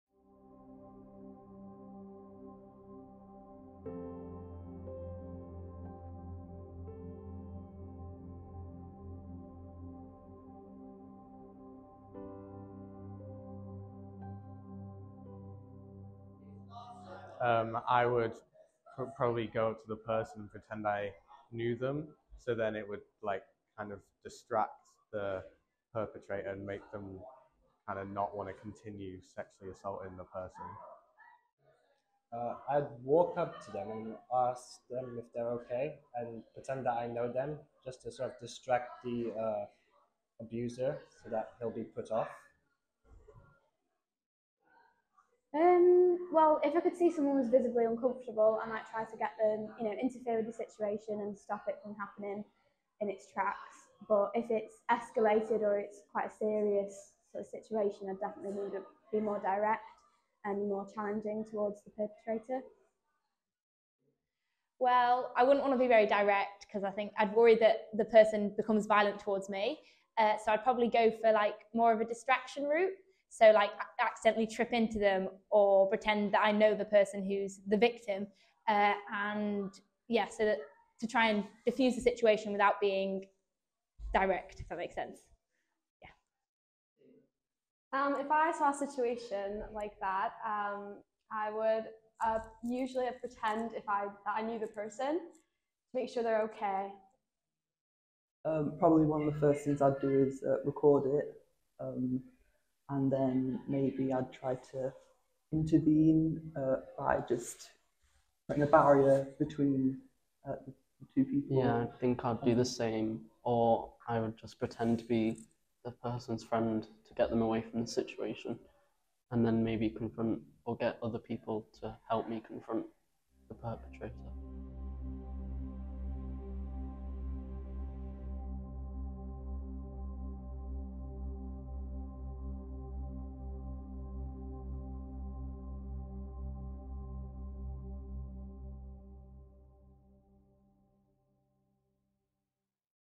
What would you do? Eaton Bank Academy
In this film Eaton Bank Academy students say what they would do if they witnesses sexual harassment on public transport, and how they would use the 'five Ds' to help alleviate the situation.